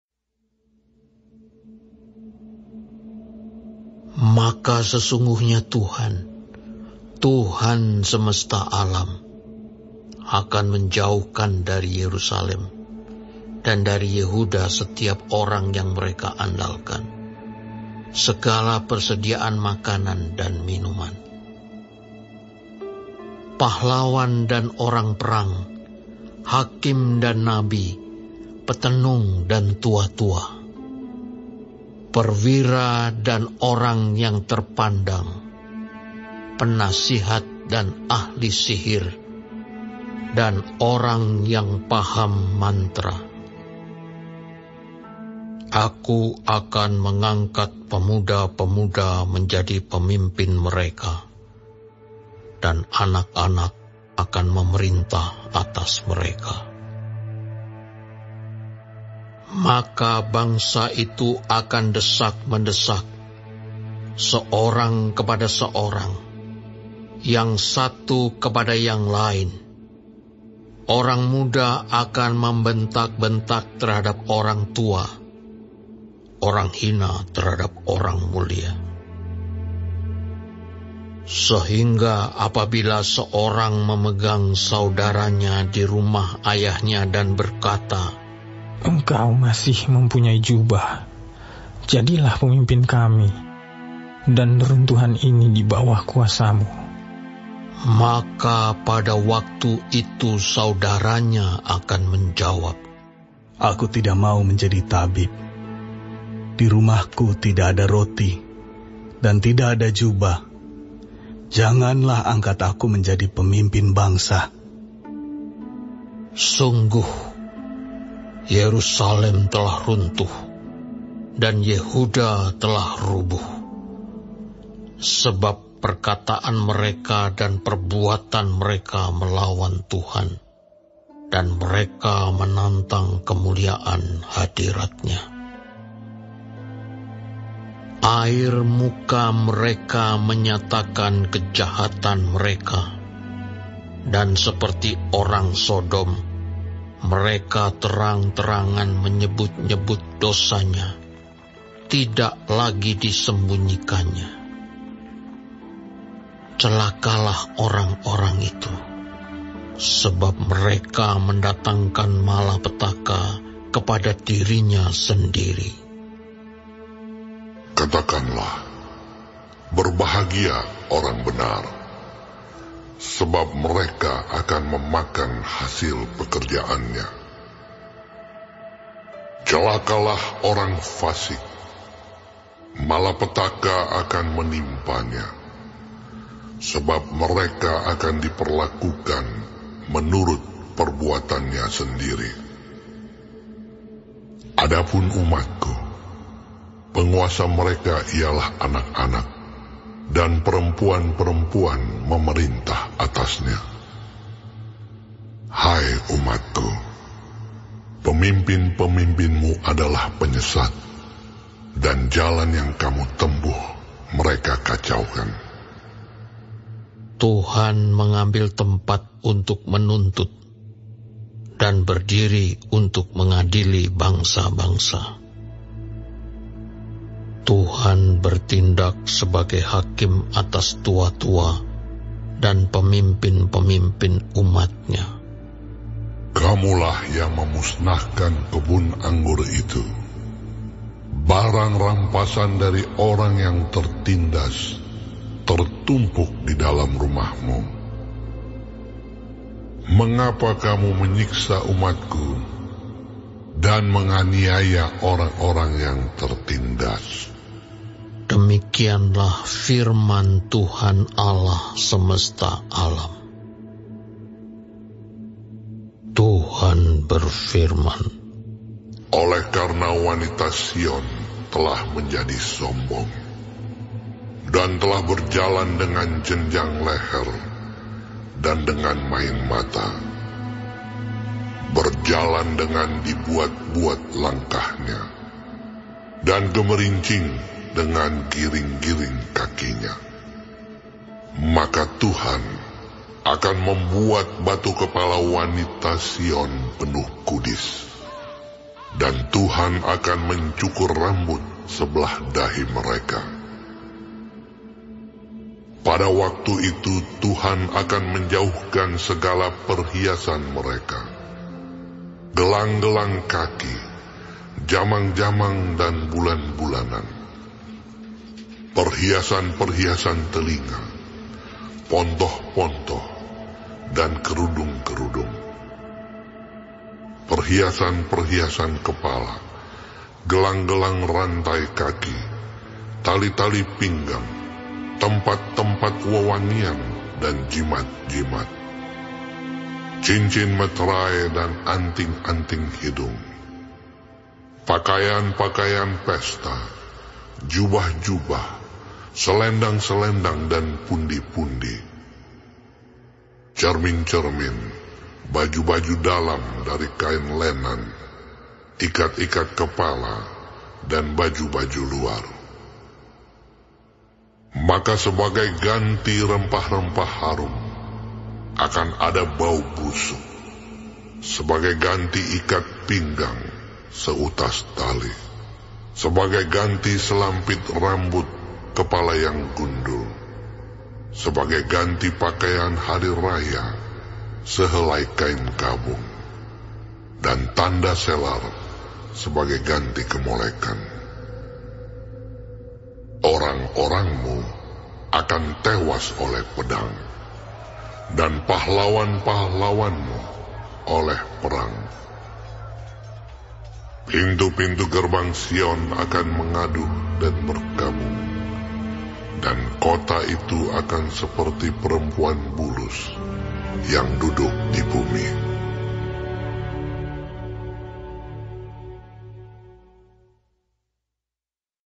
Yesaya 3 [23.0682/66.1189]
BAGIAN PERTAMA (Bab 1-39)

Bab 1-12 Nubuat tentang Yehuda dan Yerusalem

* Hukuman Tuhan terhadap orang-orang yang menyesatkan bangsa itu.
* Hukuman Tuhan terhadap wanita-wanita Sion yang sombong.